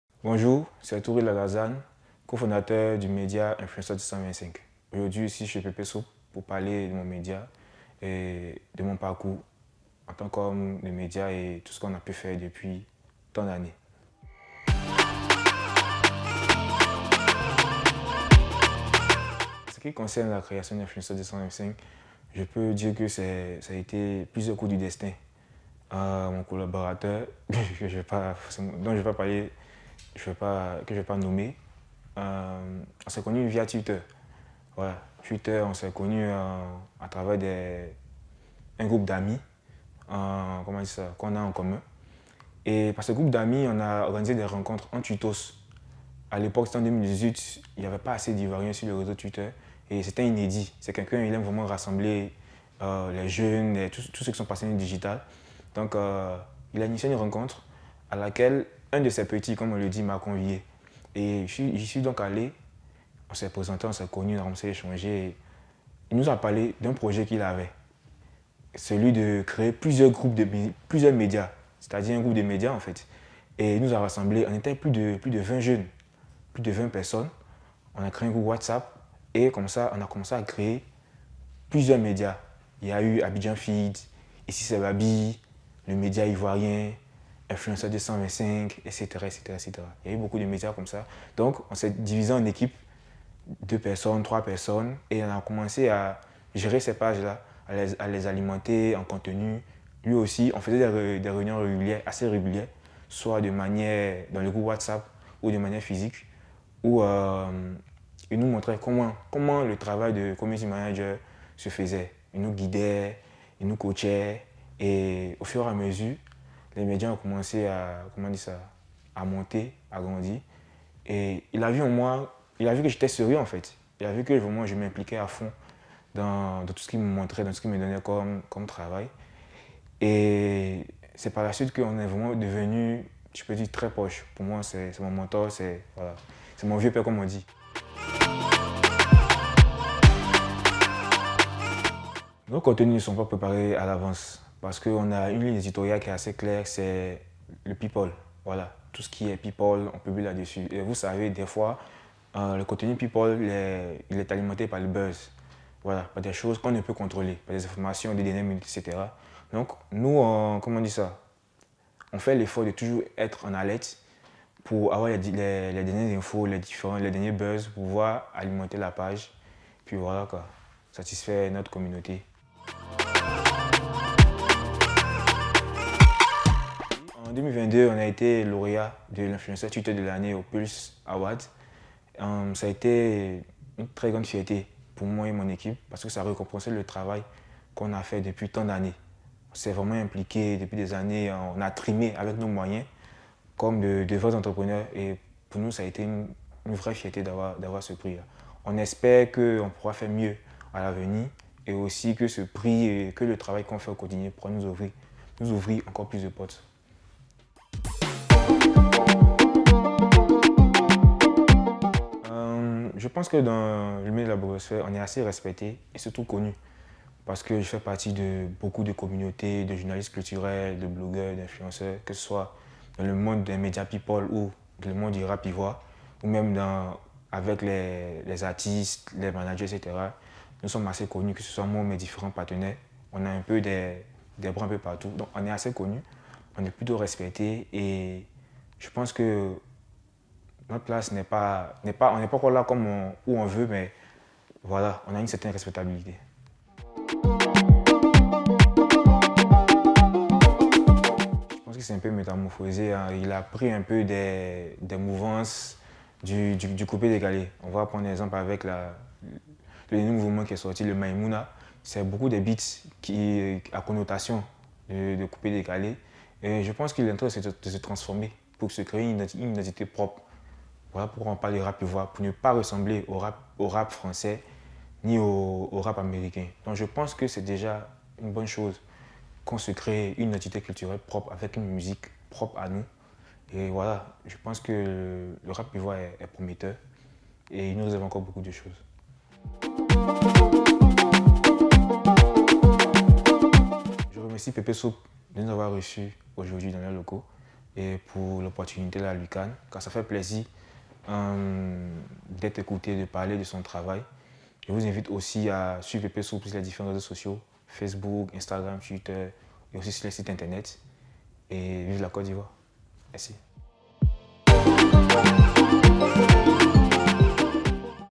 Interview exclusive de Lagazane Touré, le co-fondateur d'INFLUENCEUR 225 : tout savoir sur le média
Interview exclusive de Lagazane Touré, le co-fondateur de @influenceur225  : tout savoir sur le média